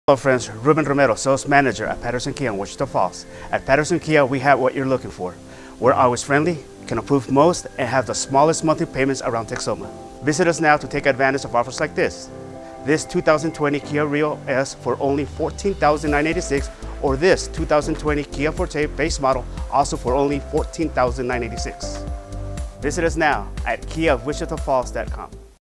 Patterson KIA - Feb 2020 - English Ad
Patterson KIA in Wichita Falls, TX - Swing by to see what sort of deals we have running on new model & clearance model units on the lot!